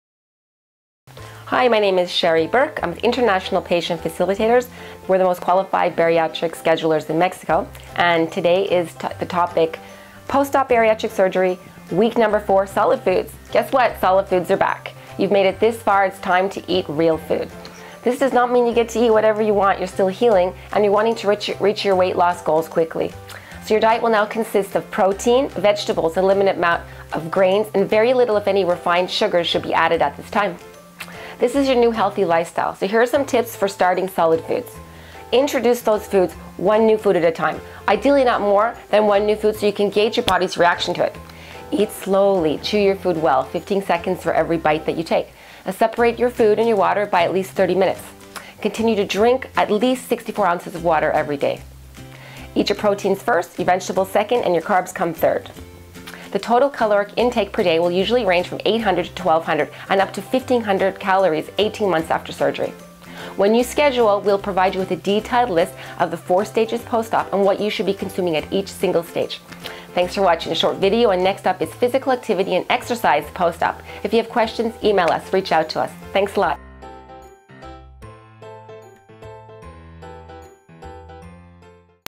How to "Bariatric Series" ( Post op Diet Week 4 )
Week Four Bariatric post op diet and what is to be consumed during the fourth of four phases / post op bariatric surgery with Sheri Burke and International Patient Facilitators.